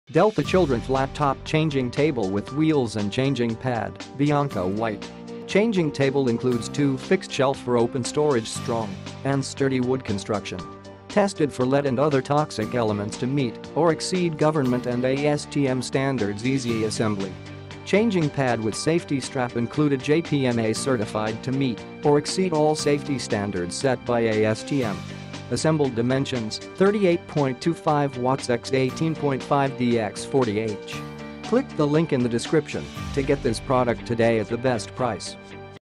Delta Children Flat Top Changing Table with Wheels and Changing Pad, Bianca White
Delta Children Flat Top Changing Table with Wheels and Changing Pad, Bianca White Changing table includes two fixed shelf for open storage Strong & sturdy wood construction
Tested for lead and other toxic elements to meet or exceed government and ASTM standards Easy assembly
Changing pad with safety strap included JPMA certified to meet or exceed all safety standards set by ASTM
Assembled Dimensions: 38.25""W x 18.5""D x 40""H"